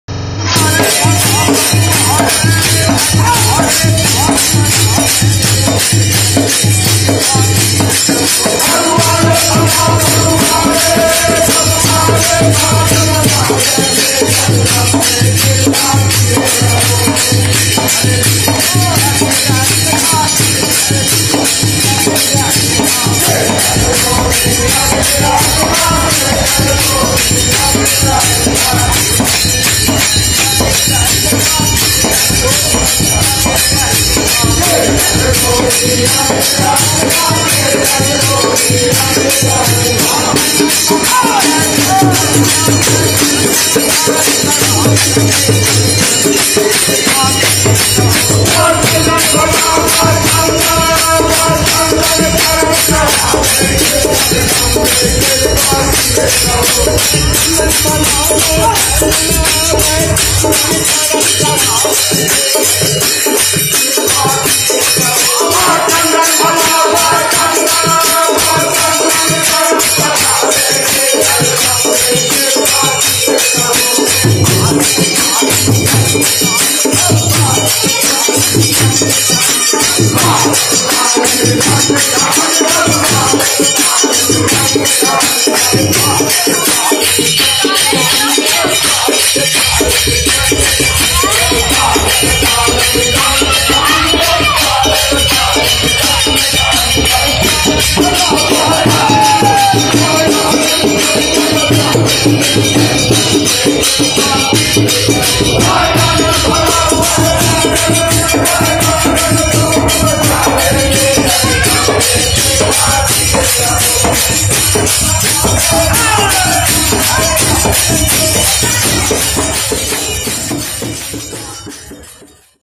Barahiya Jagdamba Mandir- माता के प्रांगन में होली गीत गाते श्रद्धालु
बड़हिया की विख्यात माँ बाला त्रिपुर सुंदरी मंदिर में होली के अवसर पर चली आ रही परम्परा के अनुसार माता के दरबार में होली गीत गाते श्रद्धालु।